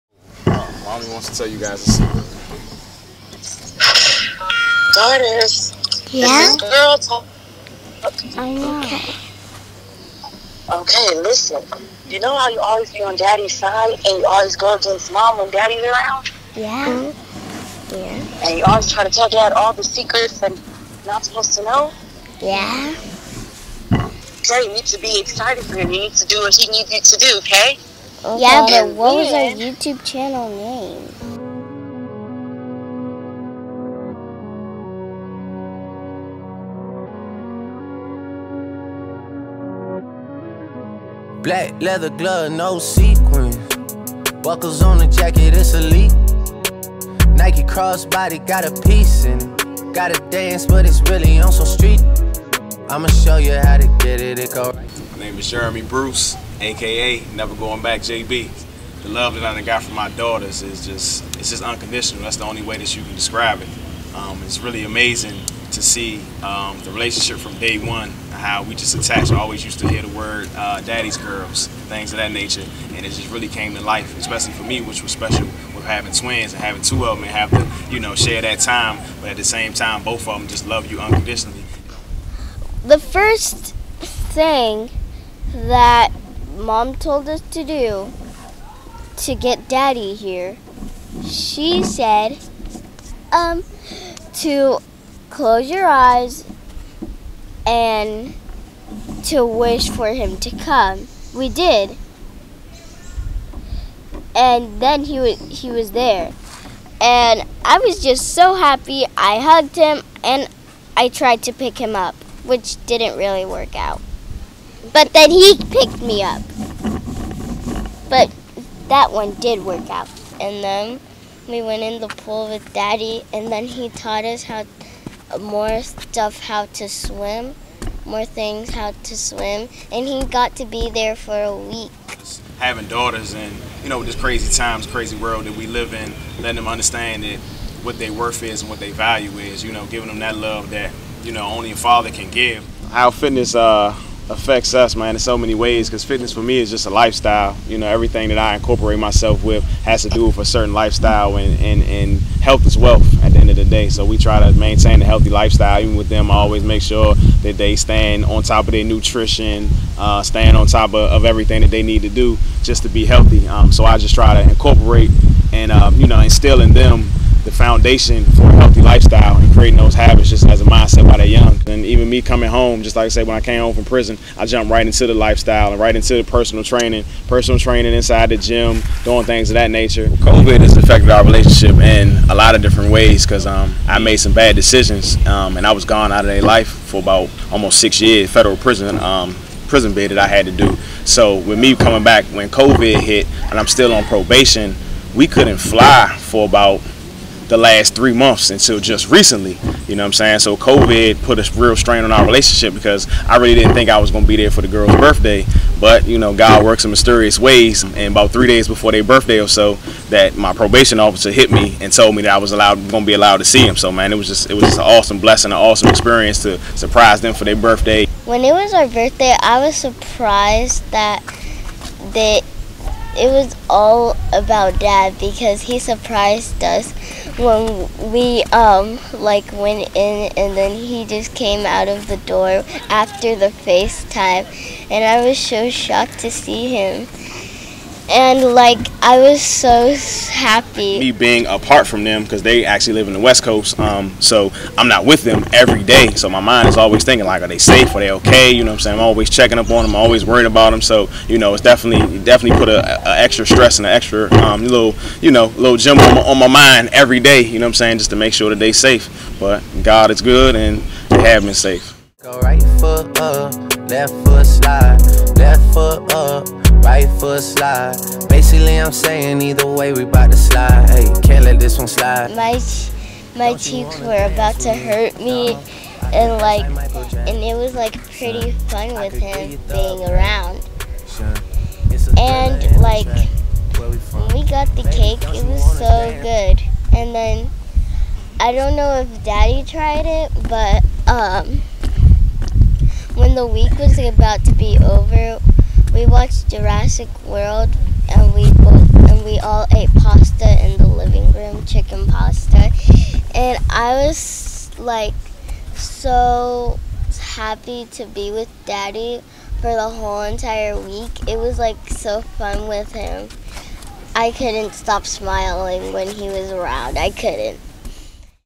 “A FATHER’S LOVE” Series 1: Ep 2
Jeremy, Jereny andJeremya Bruce.A father, not just on Father's Day!During these times we need to come togther and show the presense of fatherhood and family. @TheHuddleFoundation @aperture_ntp @theunionmediagroup decided to start a series that captures the voices of black father's and the importance of their roles on shaping our kids future. Join us to hear these amazing stories as we dive into the journey of black fatherhood.